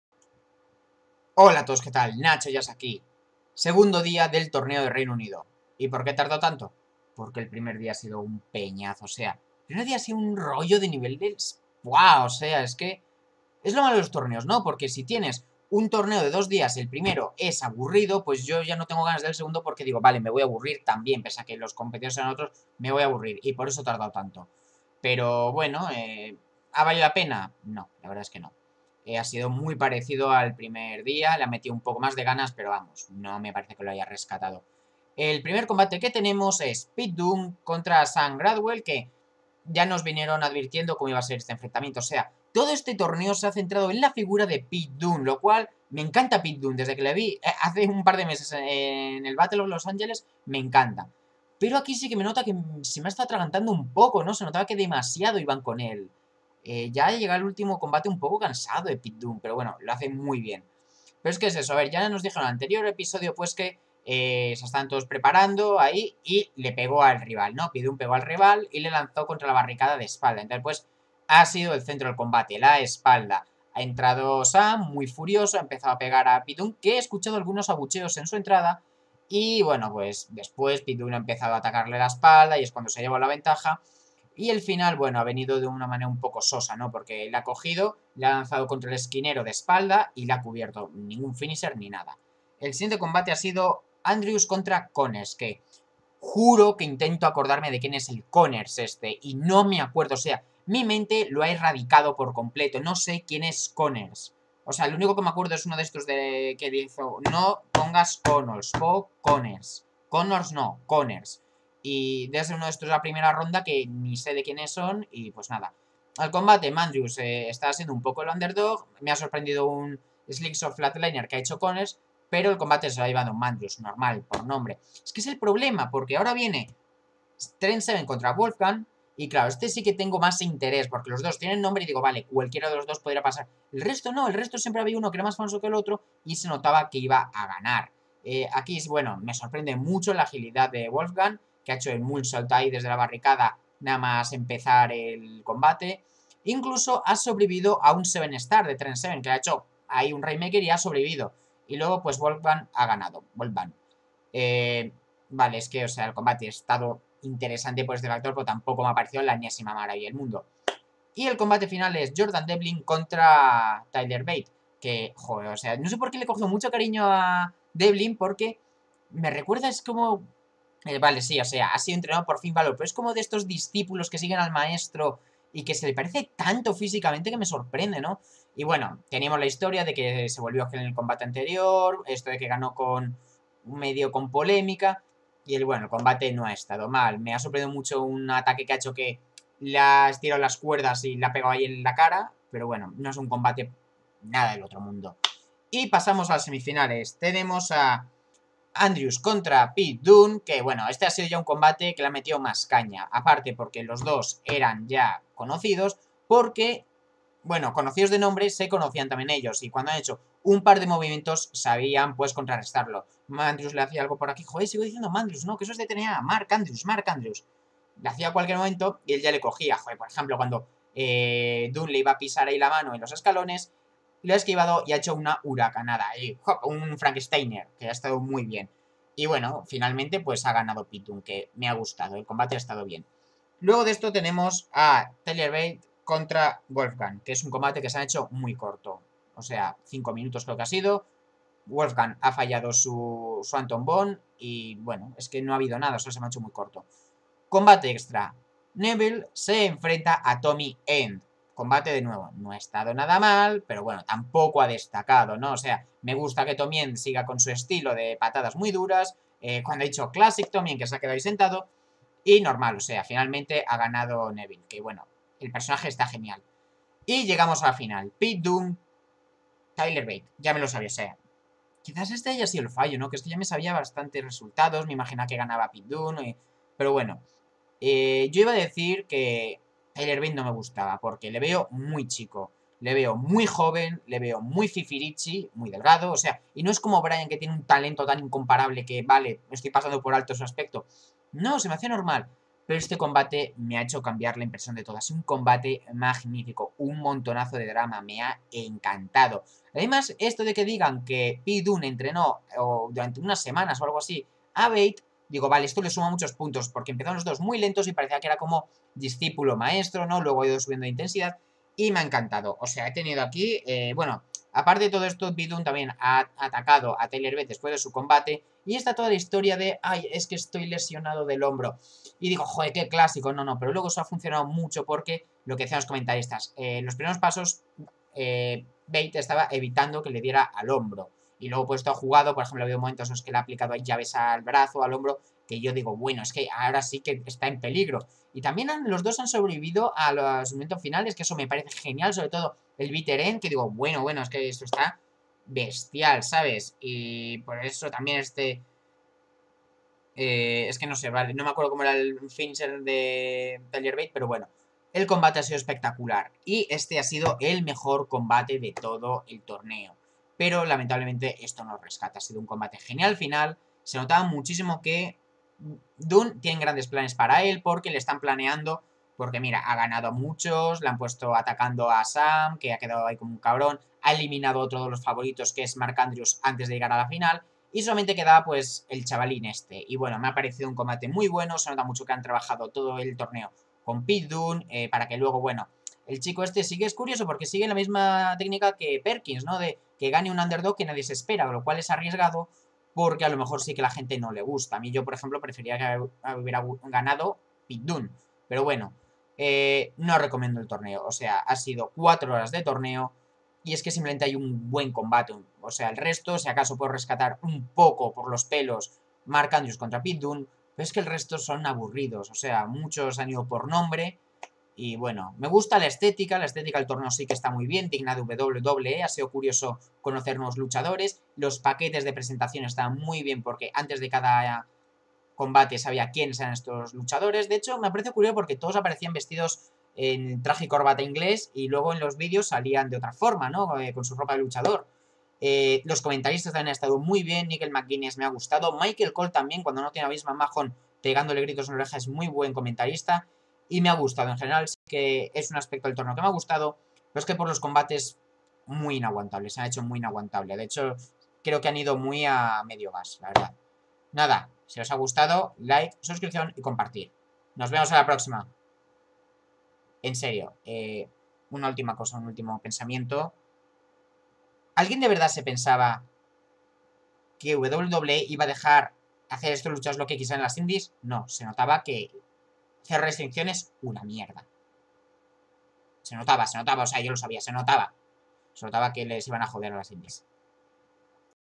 Nacho Jazz Reseña WWE UK Tournament Día 2
Según día del (aburrido) torneo de WWE en Reino Unido.